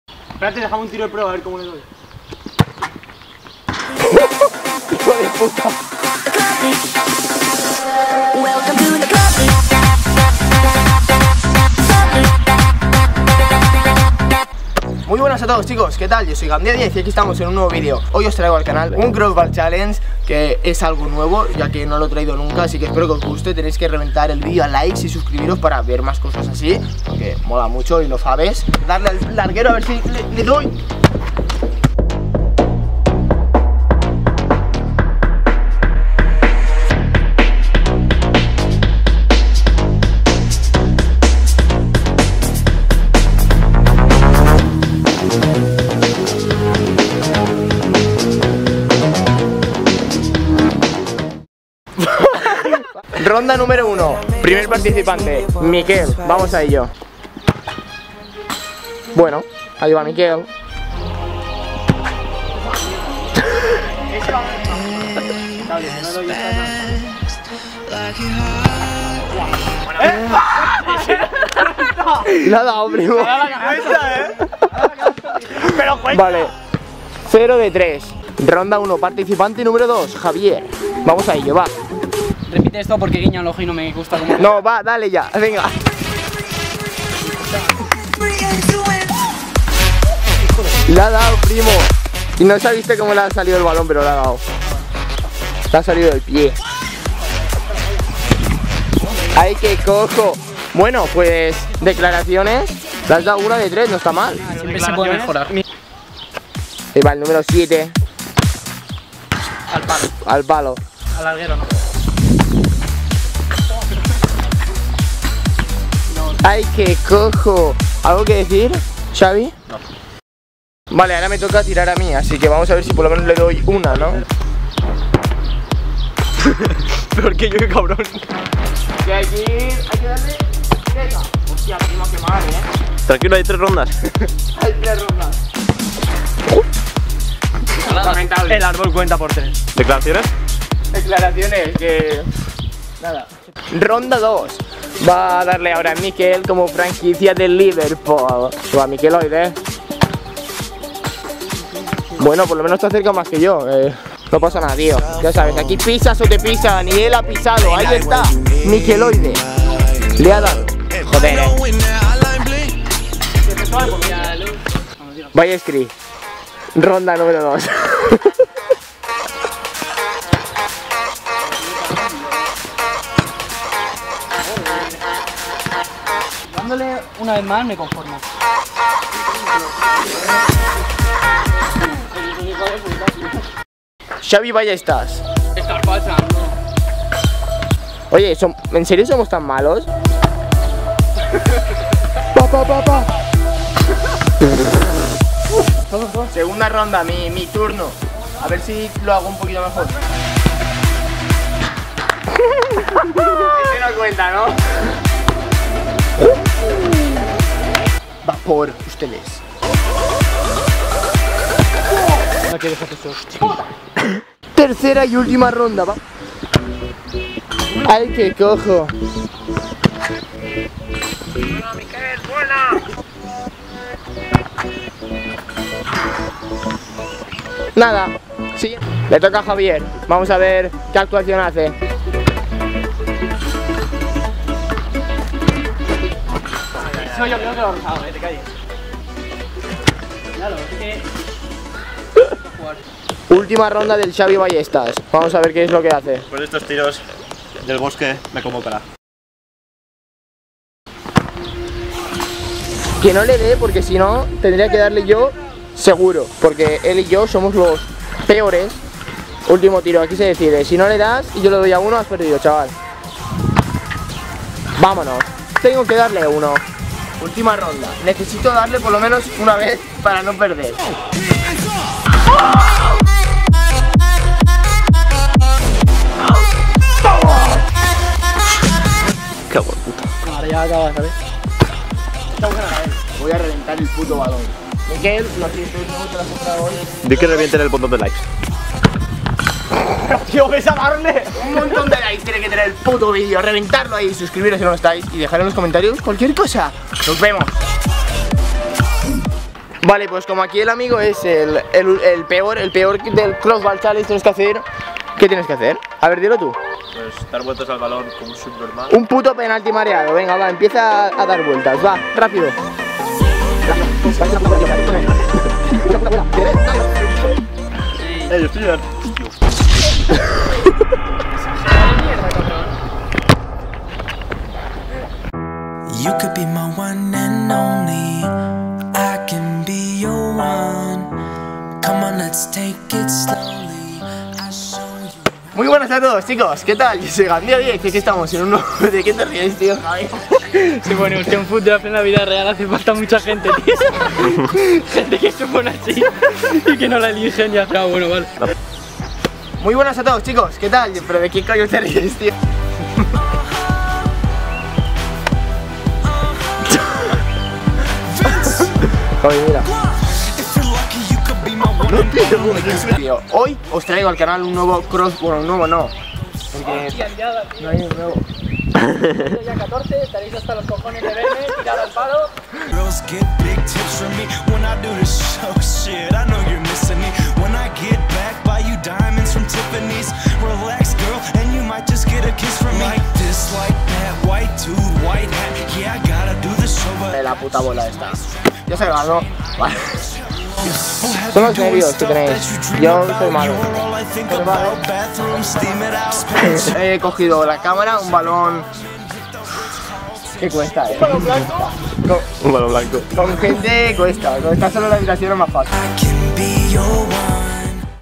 CROSSBAR CHALLENGE CON AMIGOS | FUTBOL EN LA VIDA REAL | Gandia10
Muy buenas!
Hoy os traigo al canal algo nuevo, un CROSSBALL CHALLENGE CON AMIGOS, es un reto que consiste en darle al larguero.
El reto constará de 3 rondas, en que cada participante tira 3 veces por ronda.
¿QUIÉN GANARÁ?
Comenta cualquier cosa, si tienes alguna idea de vídeo^^
0:06 Empieza la súper intro
0:14 Explicación del vídeo
0:53 Ronda 1
2:23 Ronda 2
3:35 Ronda 3
4:59 Reto del perdedor, Penalty Mareado
5:25 TOMAS FALSAS

► AMIWITOS :
( GENTE MOLONA )